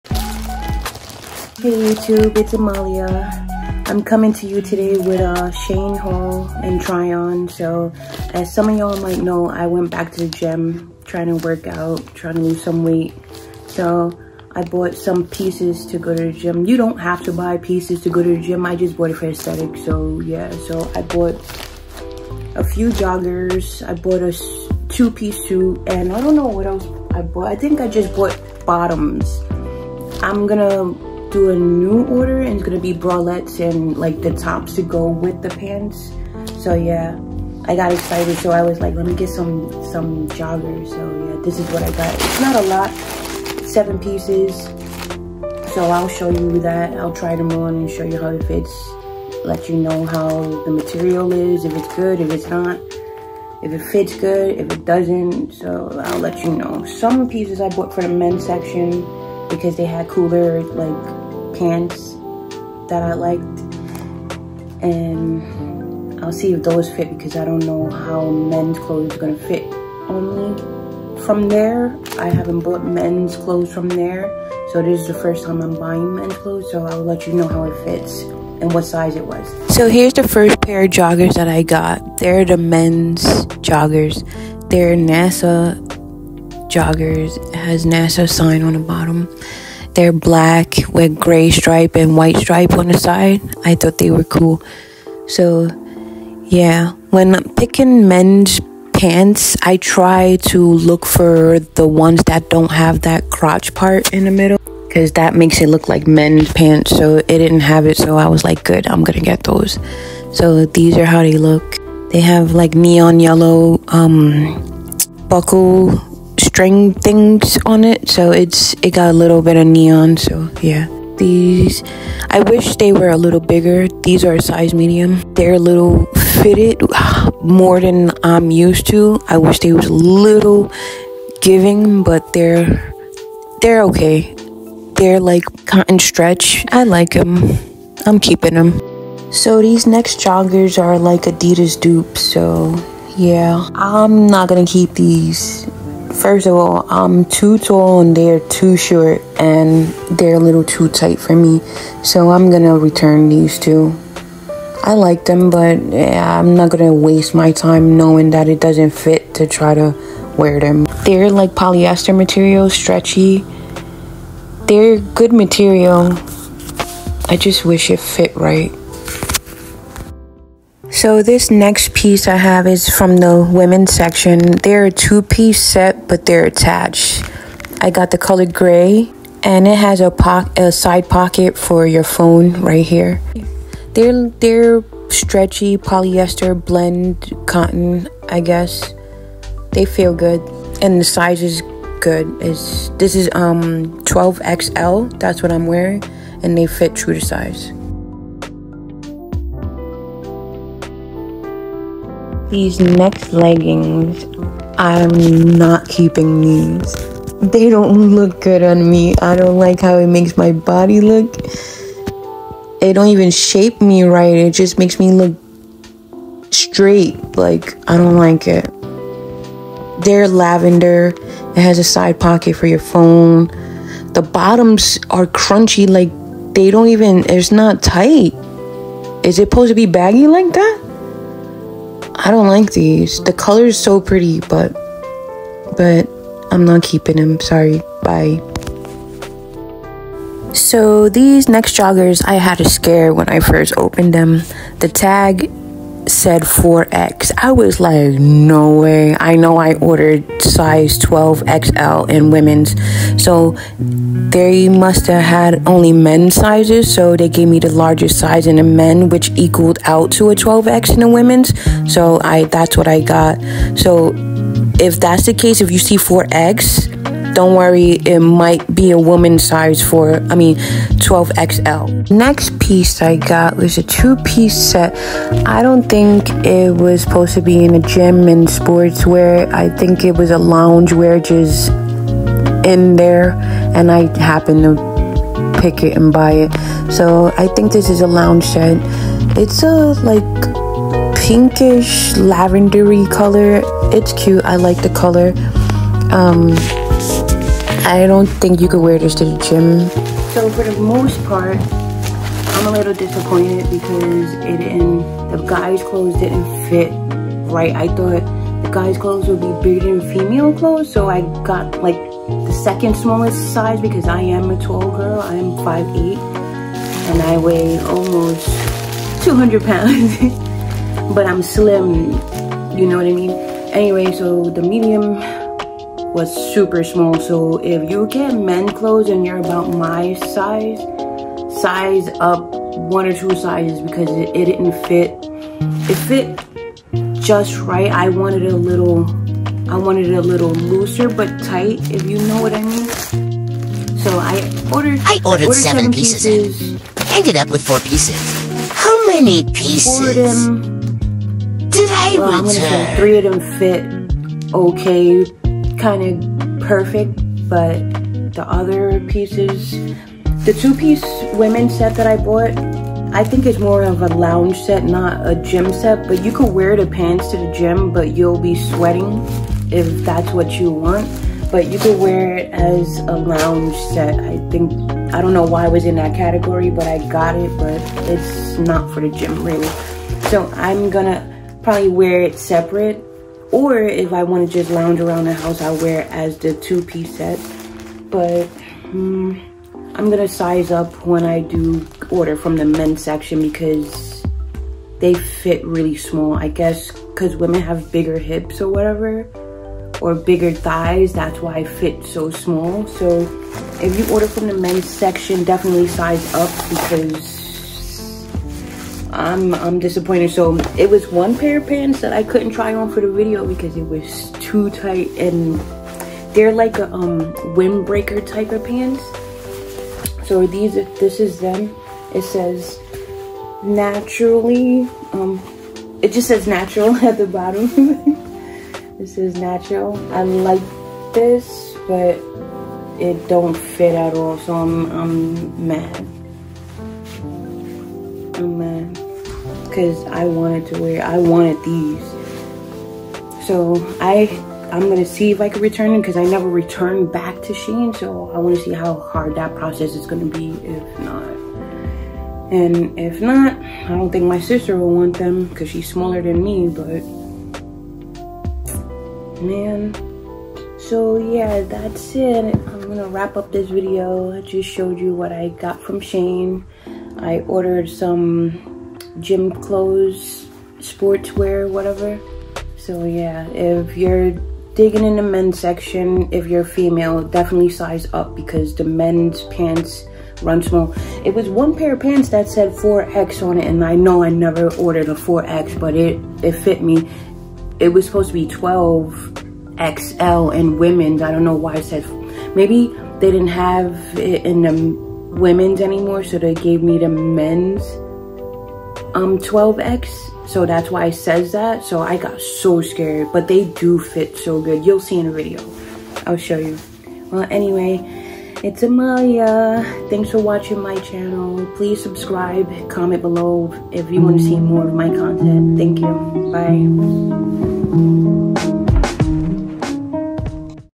MY SHEIN HAUL ( Work out Bottoms, jogger)
My SHEIN HAUL 🛍️🛍️

I order from SHEIN some gym bottoms and tried them on!

Some were okay others weren’t stay tune and see.